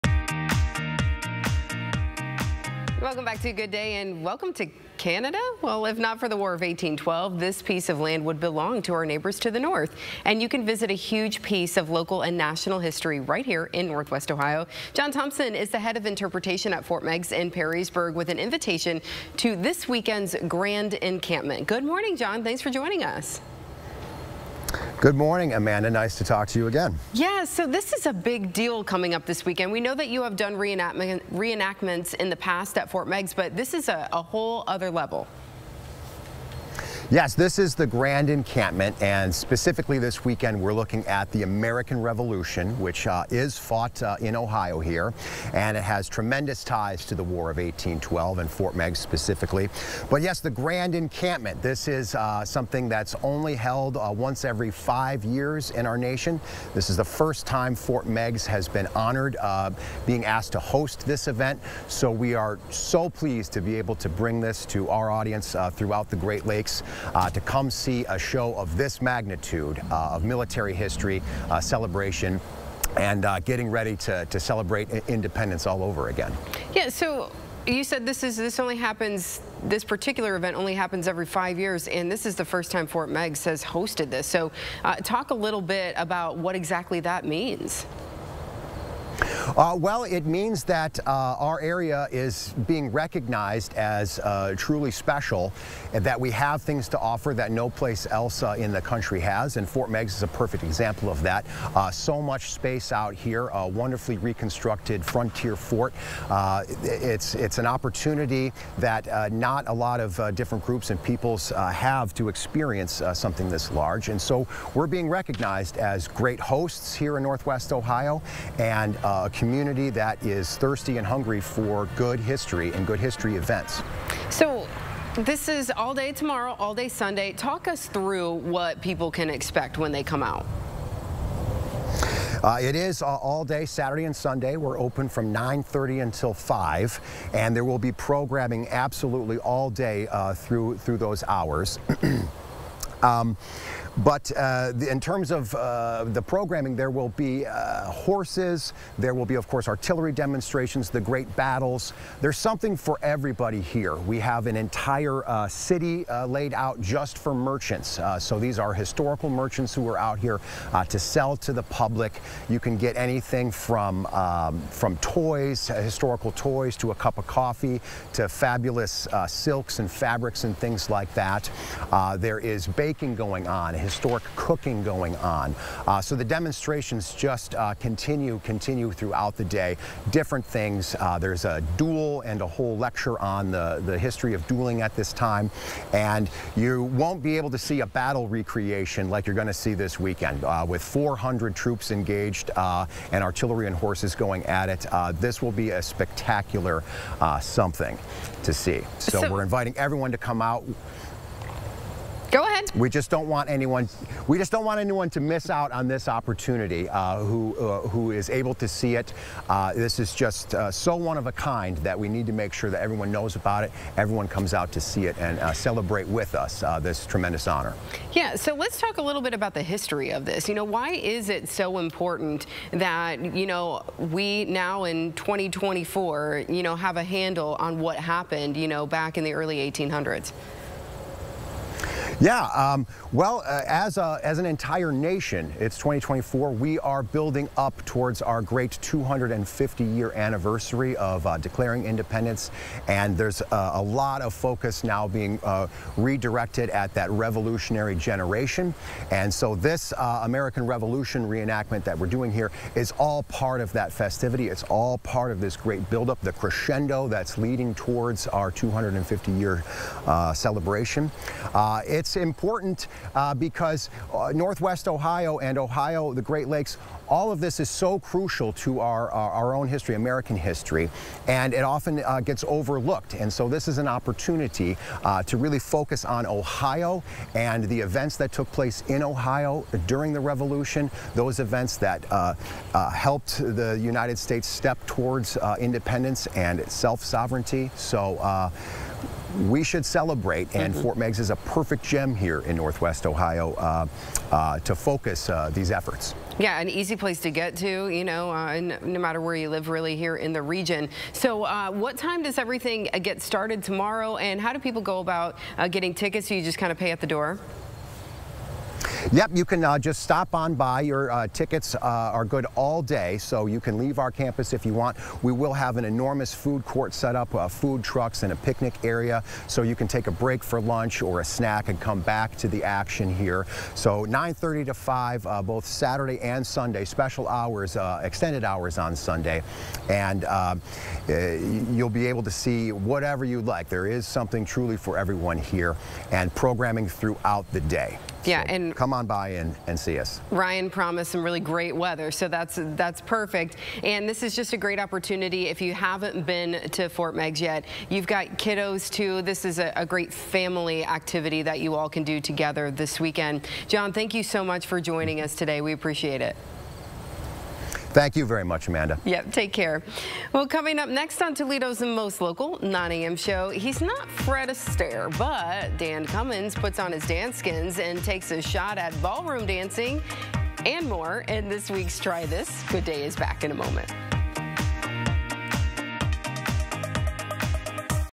A step back in time in your backyard: For Meigs Grand Encampment | Good Day on WTOL 11
John Thompson, the head of interpretation at Fort Meigs in Perrysburg, talks this weekend's Grand Encampment.